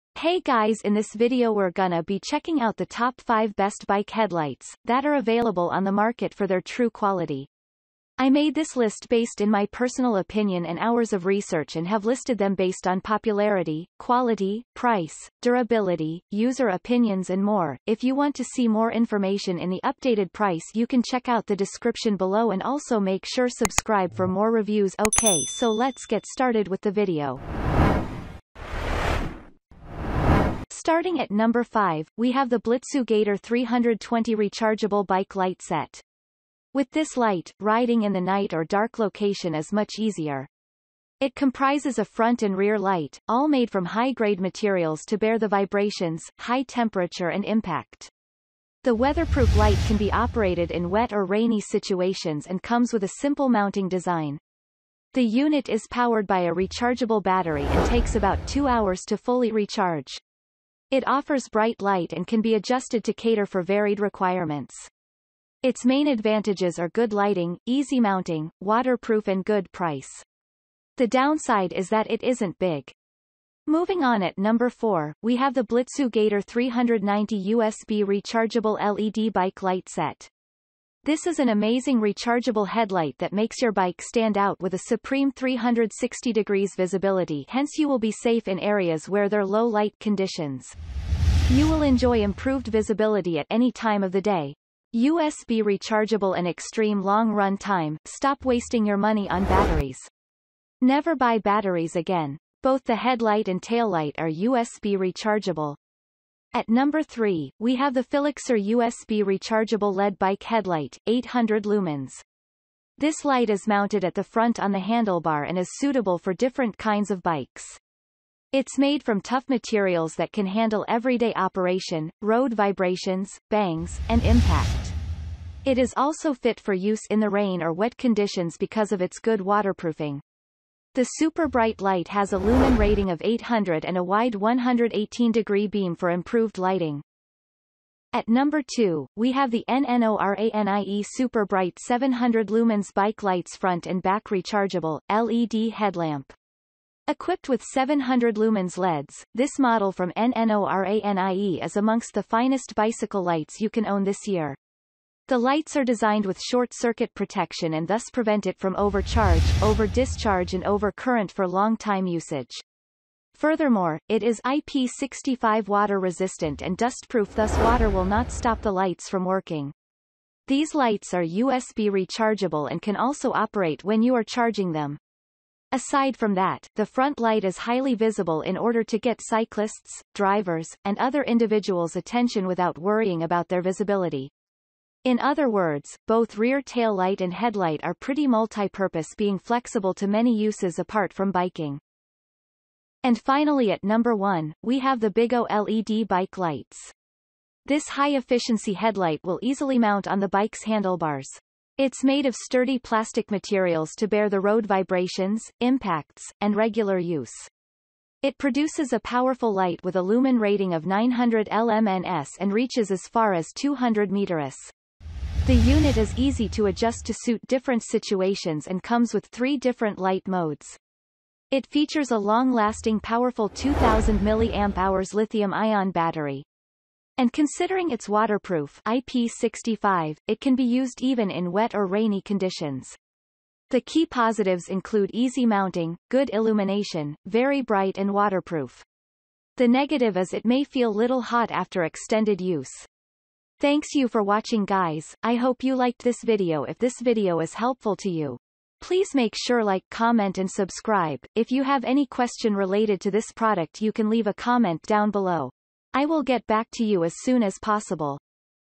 Top 5 Best Bike Headlights in 2024 – Reviews
▶️ 5.  Blitzu Gator 320 Rechargeable Bike Light Set

▶️ 4.  BLITZU Gator 390 USB Rechargeable LED Bike Light Set

▶️ 3.  Fillixar USB Rechargeable Led Bike Headlight, 800 Lumens

▶️ 2.  N N.ORANIE Super Bright 700 Lumens Bike Lights Front and Back Rechargeable, LED Headlamp

▶️ 1.   BIGO LED Bike